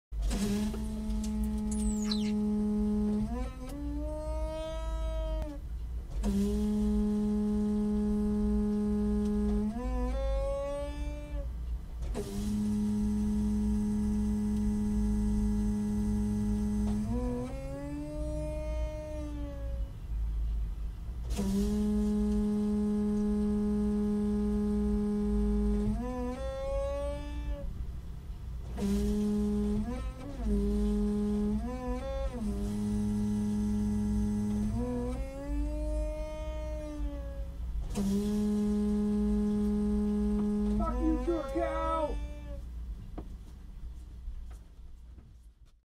Taming the Sewer Cow
Does your RV plumbing groan like a sick cow? Can't figure out that whining noise you get with running water? You may be surprised...find out how to fix it